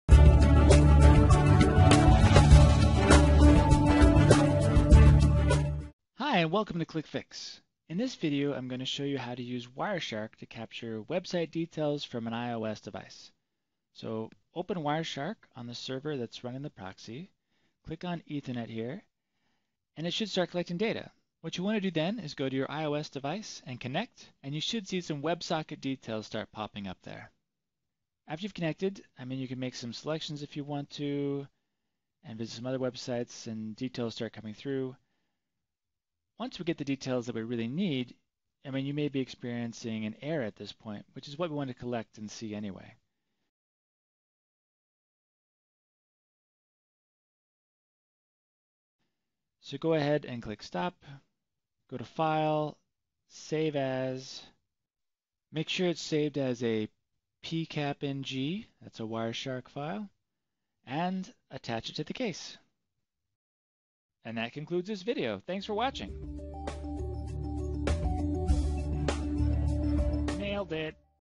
Qlik Fix: Websockets with Wireshark
This video demonstrates how to capture websocket details from an iOS device (iPhone, iPad) to help the investigation of a Qlik Support case. These steps are only applicable when using HTTP, not HTTPS.